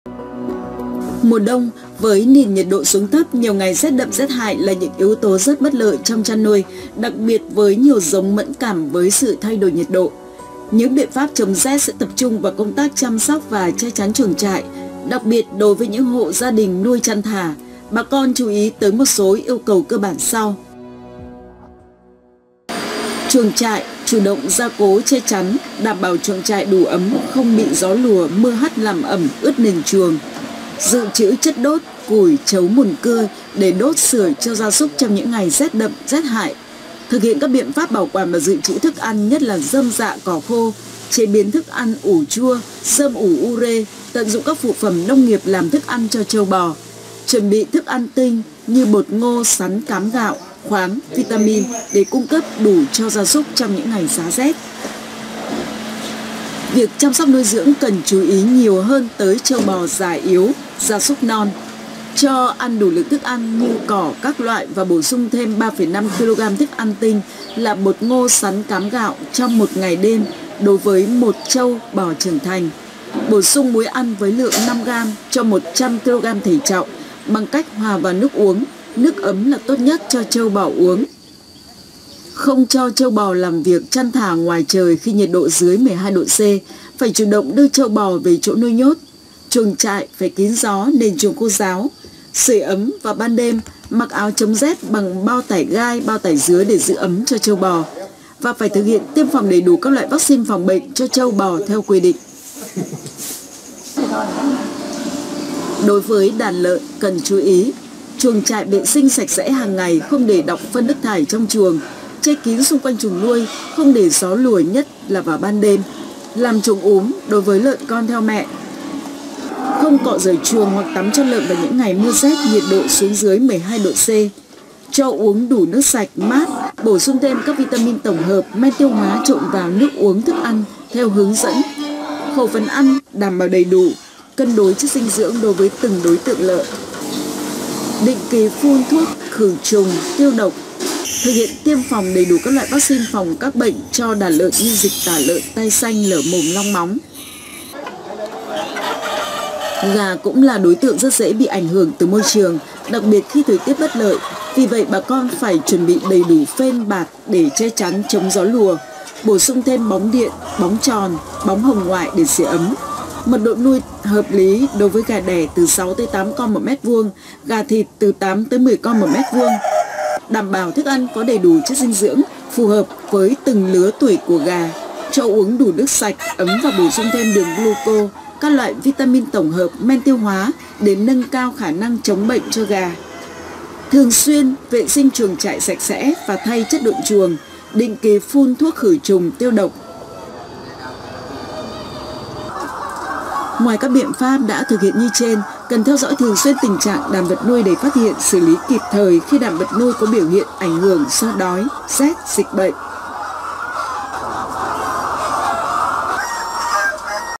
Cách chăm sóc gia súc, gia cầm mùa lạnh
để phòng chống rét cho đàn vật nuôi bà con cần áp dụng tốt các giải pháp sau:
          1. Đối với chuồng trại:
Có hệ thống bạt, rèm che chắn chuồng, bảo đảm chuồng trại kín, ấm, khô. Đặc biệt, nền chuồng luôn khô ráo, sạch sẽ, đối với nền chuồng gà, trâu bò cần rải thêm lớp trấu, rơm khô để giữ ấm chân và tăng nhiệt độ trong chuồng.
Có thể chăn nuôi gà trên nền đệm lót vi sinh để nâng cao nhiệt độ chuồng nuôi giúp sưởi ấm cơ thể vật nuôi.
          2. Thời điểm chăn thả gia súc, gia cầm:
Bà con cần thường xuyên theo dõi bản tin dự báo thời tiết để có kế hoạch chăn thả gia súc, gia cầm hợp lý. Hạn chế việc chăn thả gia súc, gia cầm khi nhiệt độ ngoài trời dưới 150C (đặc biệt là gia súc, gia cầm nhỏ). Những ngày rét đậm, rét hại dưới 120C thì không chăn thả mà nhốt vật nuôi trong chuồng để tiện chăm sóc, quản lý và tăng cường sưởi ấm vật nuôi bằng bóng điện sợi đốt, bóng hồng ngoại hoặc đốt củi, bếp than để sưởi (chú ý khi đốt lửa sưởi phải có đường ống dẫn khói thải ra ngoài chuồng, tránh hiện tượng ngạt, ngộ độc vật nuôi và vị trí đốt tránh xa chất độn chuồng, bạt che để phòng bén lửa gây cháy), đối với đàn trâu bò nên mặc áo ấm bằng bao tải gai, bao tải dứa, chăn, áo cũ,…để chống rét.
3. Chăm sóc nuôi dưỡng:
                + Đối với gia cầm:
               + Thực hiện tốt quy trình úm cho gia cầm nhỏ, lưu ý đối với các giống gà mọc lông chậm hoặc ít lông (gà chọi, gà Mía, Đông Tảo,…) khả năng chịu lạnh kém cần có biện pháp chống rét thích hợp (che chắn chuồng tránh gió lùa trực tiếp, tăng nguồn nhiệt bằng cách bổ sung thêm bóng điện, bóng hồng ngoại; Không thả gia cầm ra vườn, đồi trong những ngày có rét đậm, rét hại,…).
Cho gia cầm uống nước ấm, bổ xung chất điện giải, B. comlex, men tiêu hóa nhằm nâng cao sức đề kháng vật nuôi.
                + Đối với chăn nuôi lợn:
                     Nên có chuồng úm đối với lợn con theo mẹ. Không cọ rửa chuồng hoặc tắm cho lợn vào những ngày mưa rét nhiệt độ xuống dưới 120C. Cung cấp khẩu phần ăn đảm bảo đầy đủ, cân đối chất dinh dưỡng đối với từng đối tượng lợn, giai đoạn nuôi.
Cho uống đủ nước sạch, bổ sung thêm các Vitamin tổng hợp, men tiêu hoá trộn vào nước uống, thức ăn.
               + Đối với trâu, bò:
                    Cung cấp đủ thức ăn thô xanh cho trâu bò, với định mức bằng 10% trọng lượng cơ thể. Ví dụ bò nặng 300 kg, cần cho bò ăn 30 kg cỏ xanh một ngày. Đồng thời bổ sung thêm 0,5 – 1kg thức ăn tinh/con/ngày (bột ngô, bột sắn, cám gạo,.. ) để trâu bò có đủ năng lượng chống rét.
Đối với những ngày rét đậm cần đun nước ấm cho trâu bò uống, bổ sung thêm muối ăn với định lượng 5gam/100kg trọng lượng cơ thể nhằm tăng khả năng trao đổi chất và nâng cao sức đề kháng cơ thể.
                     Cung cấp thức ăn xanh ủ chua cho trâu bò như ủ chua cỏ voi, VA06, thân cây ngô,.. với lượng 7 – 10 kg/ngày, kết hợp ăn thêm cỏ xanh, rơm nhằm nâng cao tỷ lệ tiêu hóa và đảm bảo sức khỏe cho đàn trâu bò trong mùa rét.
Ngoài các biện pháp nêu trên bà con cần đẩy mạnh công tác phòng bệnh như thực hiện tốt công tác vệ sinh tiêu độc khử trùng chuồng trại, tiêm đầy đủ các loại vacxin theo hướng dẫn của cơ quan thú y. Hàng ngày theo dõi tình trạng đàn vật nuôi để phát hiện, xử lý kịp thời những con có dấu hiệu bất thường do đói, rét, dịch bệnh.